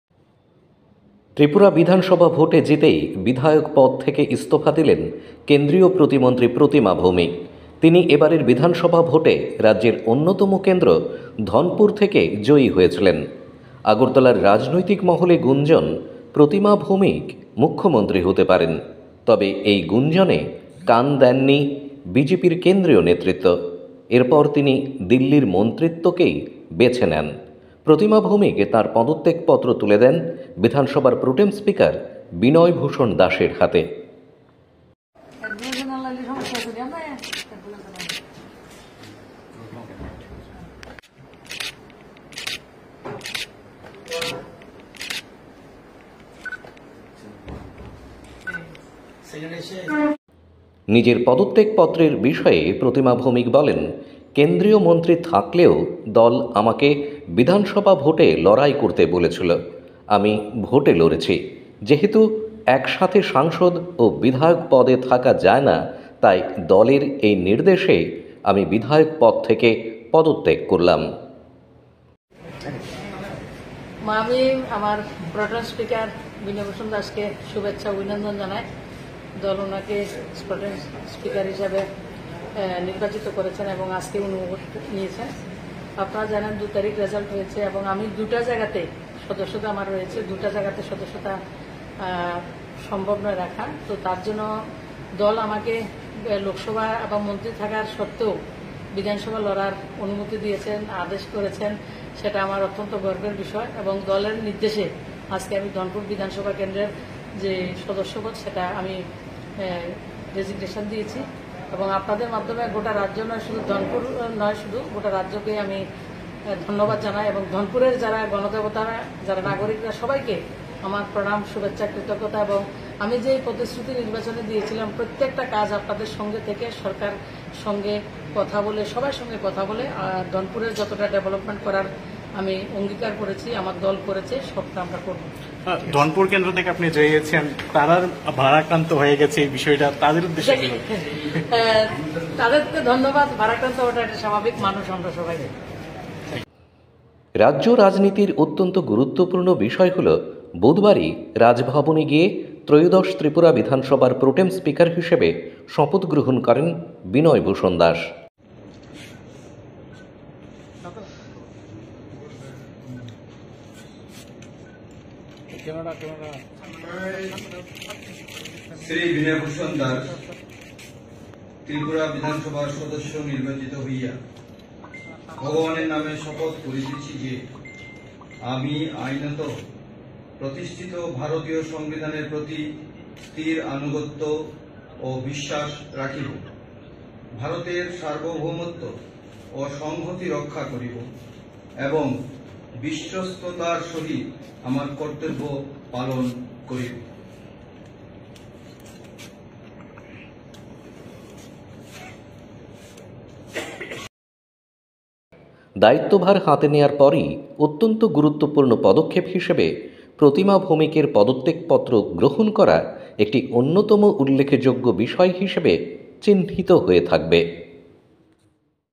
Tripura newly elected MLA Pratima Bhoumik resigns / নবনির্বাচিত বিধায়কের পদত্যাগ।
ত্রিপুরা বিধানসভা ভোটে জিতেই বিধায়ক পদ থেকে ইস্তফা দিলেন কেন্দ্রীয় প্রতিমন্ত্রী প্রতিমা ভৌমিক। তিনি এবারের বিধানসভা ভোটে রাজ্যের অন্যতম কেন্দ্র ধনপুর থেকে জয়ী হয়েছিলেন। আগরতলার রাজনৈতিক মহলে গুঞ্জন প্রতিমা ভৌমিক ষ মুখ্যমন্ত্রী হতে পারেন। তবে গুঞ্জনে কান দেয়নি BJP কেন্দ্রীয় নেতৃত্ব। এরপর তিনি দিল্লির মন্ত্রীত্বই বেছে নেন। প্রতিমা ভৌমিক তাঁর পদত্যাগপত্র তুলে দেন বিধানসভার প্রোটেম স্পিকার বিনয়ভূষণ দাসের হাতে।

ব্রহ্মকুন্ড বার্তা / Brahamakunda Bartaa
( দেশ-বিদেশ ও ত্রিপুরার সব খবরের আপডেট)